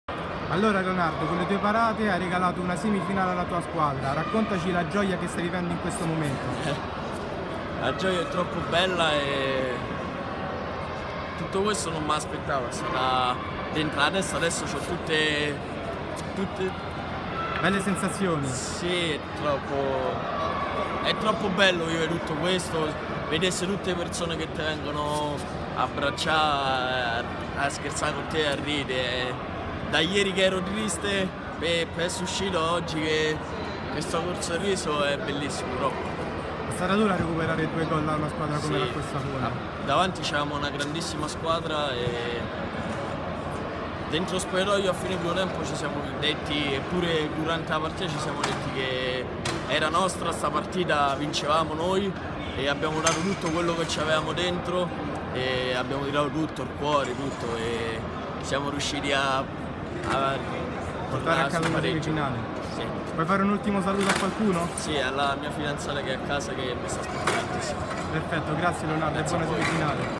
Final Eight 2016 - L'intervista a Leonardo Del Ferraro dopo Carlisport Cogianco - Acqua&Sapone
Intervista di fine partita a Leonardo Del Ferraro, l'eroe del giorno della Carlisport Cogianco